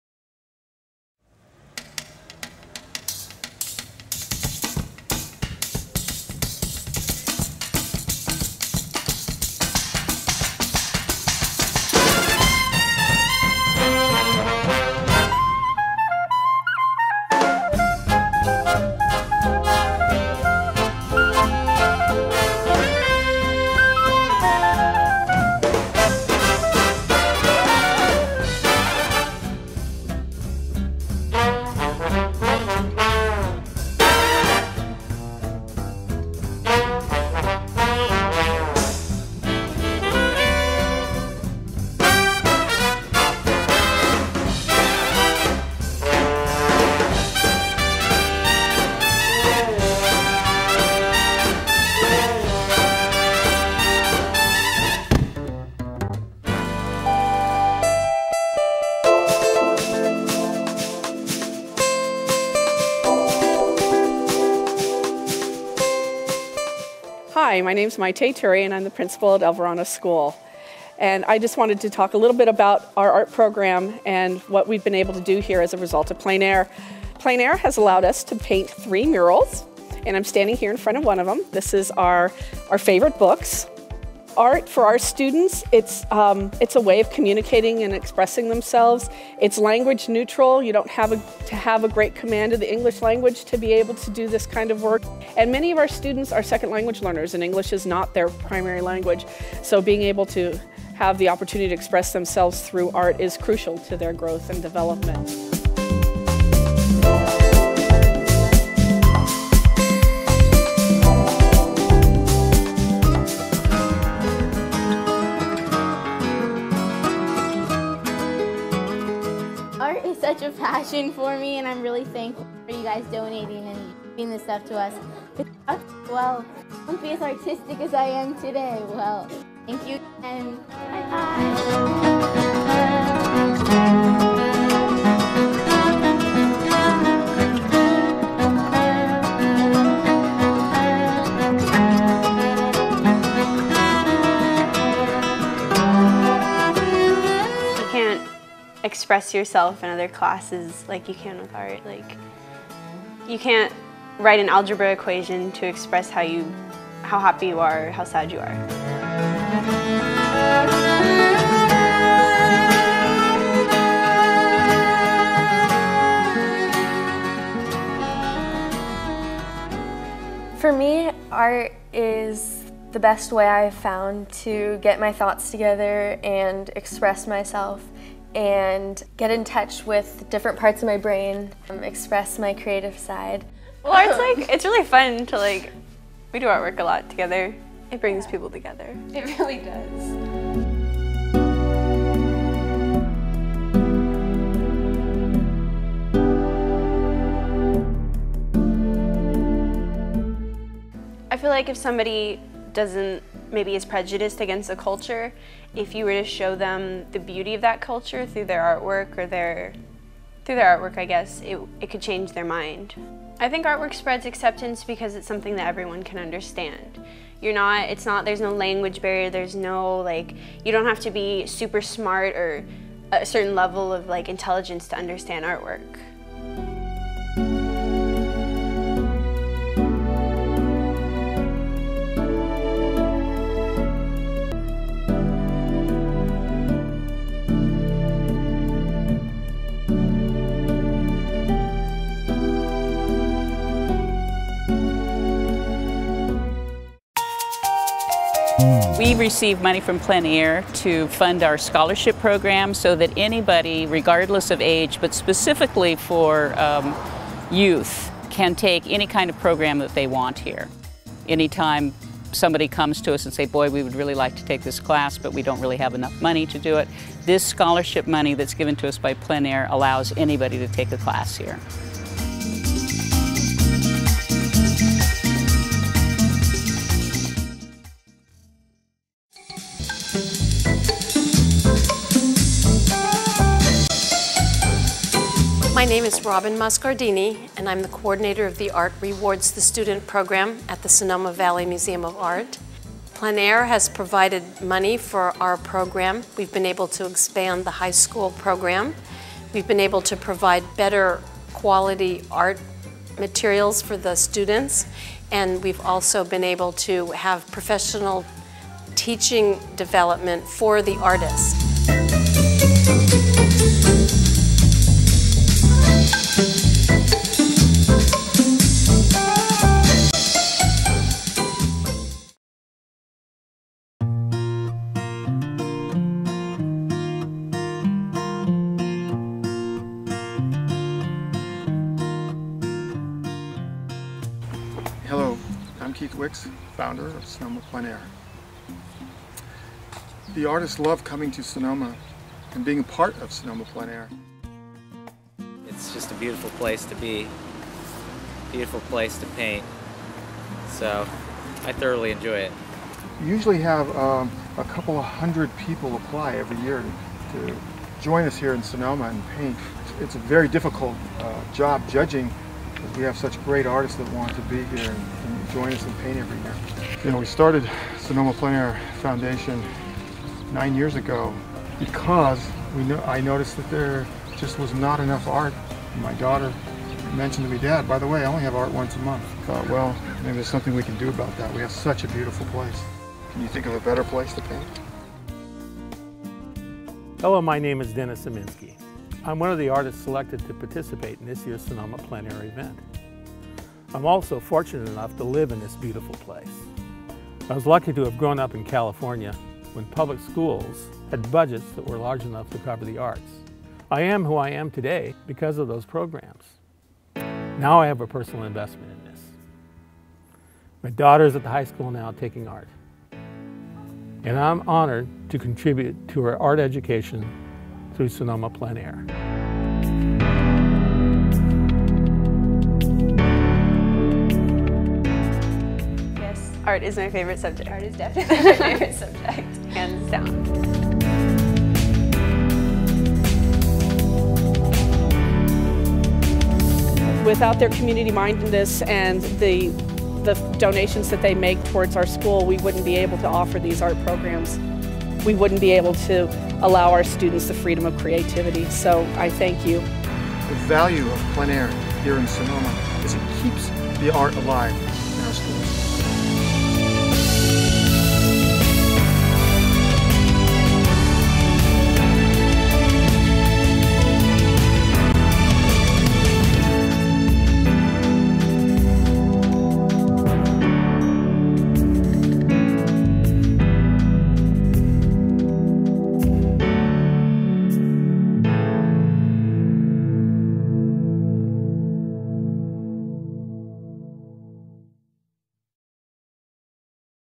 Sonoma Plein Air
This is a video I created for Sonoma Plein Air to help promote their program to potential donors.